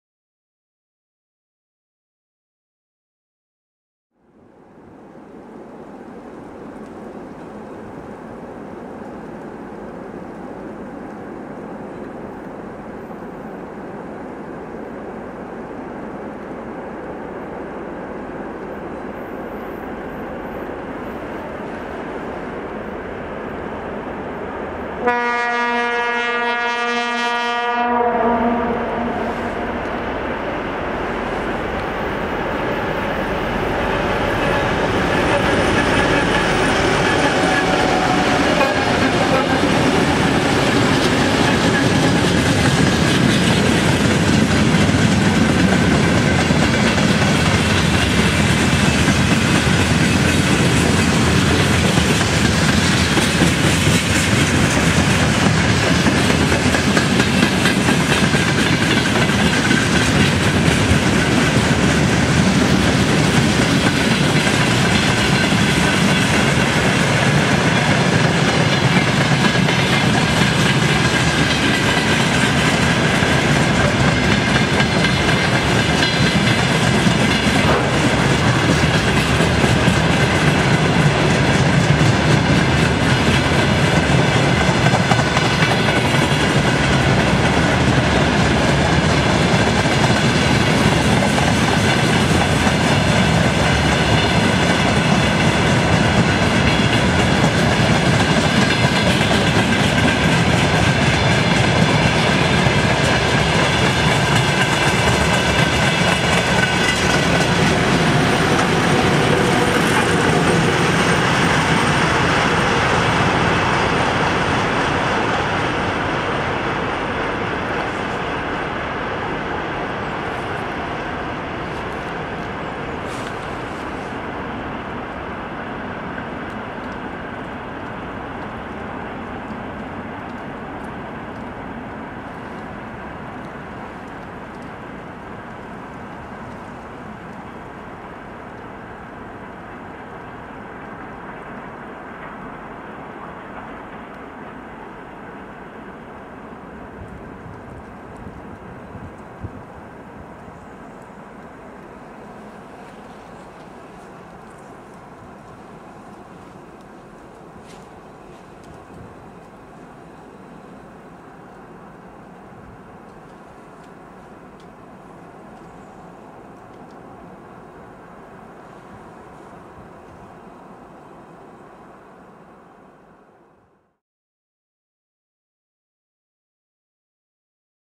5 ST44 w jednym pociągu, Malowany Most, 2007.
Na czele pociągu lokomotywy ST44-2015+2021+2031 a na końcu jako podsył lokomotywy: ST44-2003+2026.